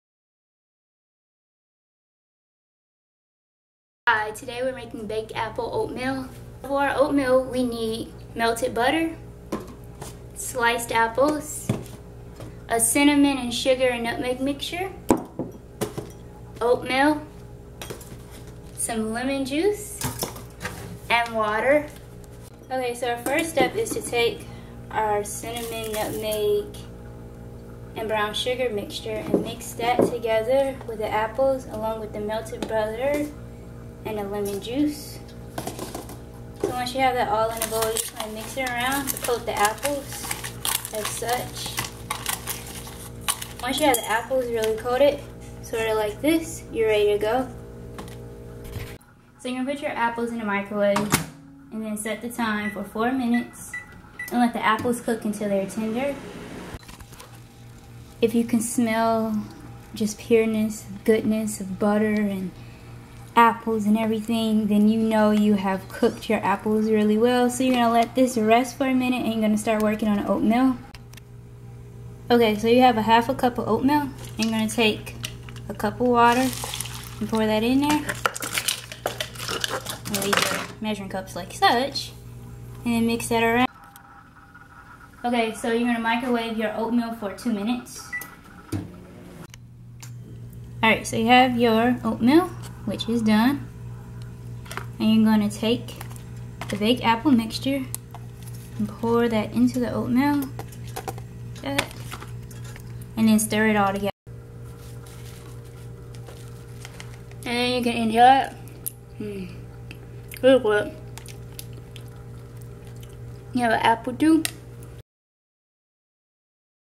Appletree: "Baked Apple Oatmeal"
Episode 1.3, of the bi-weekly web series, 31st and Snack Street, shows you how to make baked apple oatmeal.

Film Credits
Justice Plummer, on behalf of Knowledgeable Savage Films